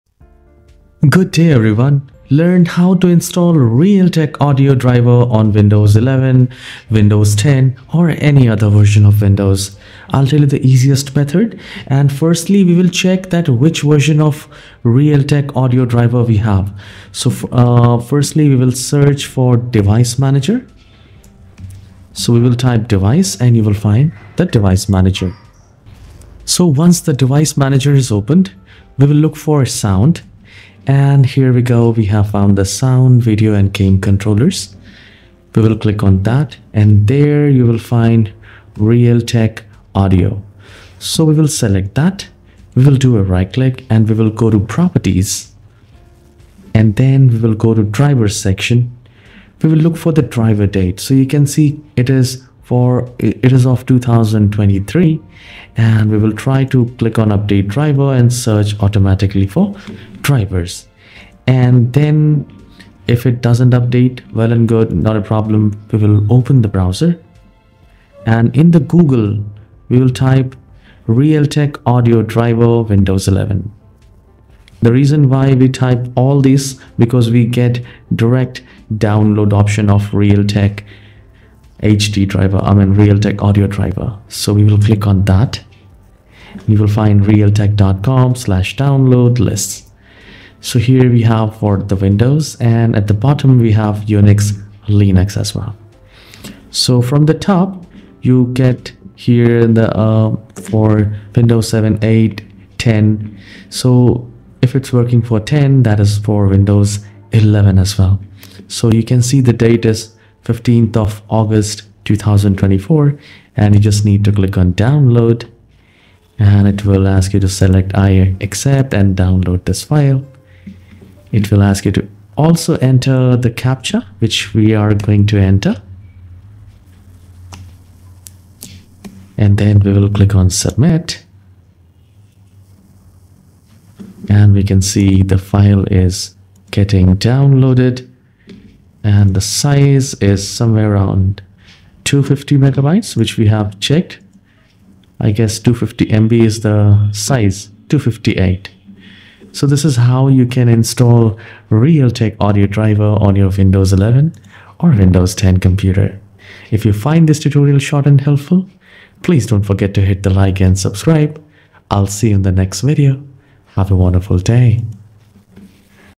How to Install Realtek Audio Driver on Windows 11/10 in 5 Minutes
Best tutorial on how to install Realtek audio driver on Windows 11 or Windows 10 step by step guide so you can fix no sound problem boost audio quality and remove crackling distortion in any laptop or desktop from HP Dell Lenovo Asus Acer MSI Samsung or Microsoft Surface

In this video you will learn how to download the official Realtek High Definition Audio driver from the manufacturer site uninstall old drivers safely and install the latest version without errors so your audio works perfectly on Windows 11 Home Pro or Windows 10 Home Pro